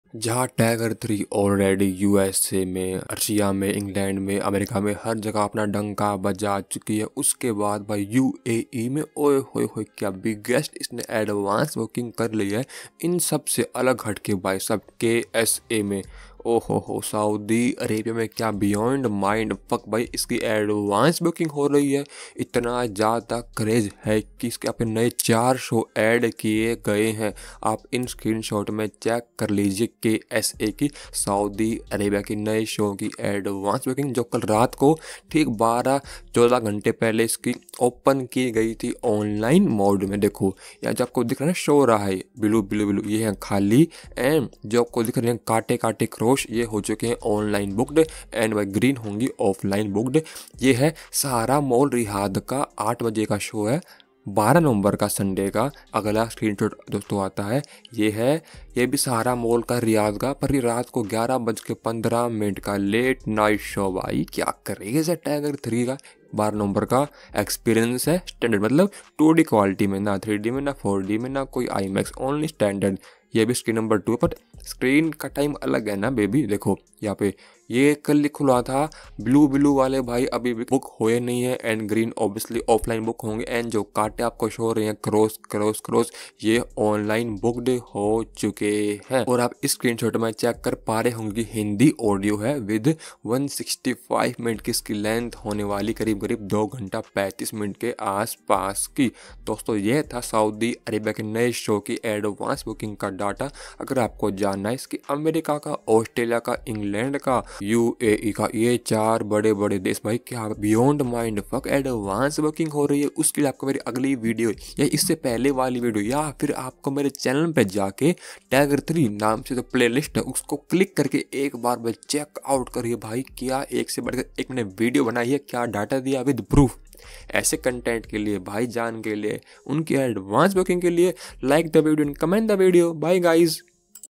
57,440 Tickets SOLD➙Last 9Hrs💥🚀Tiger 3 Advance Booking I Tiger 3 day 1 collection | Tiger 3 Review
57,440 Tickets SOLD➙Last 9Hrs💥🚀Tiger 3 Advance Booking I Tiger 3 day 1 collection | Tiger 3 Review

(Movie Recommandation + Updates)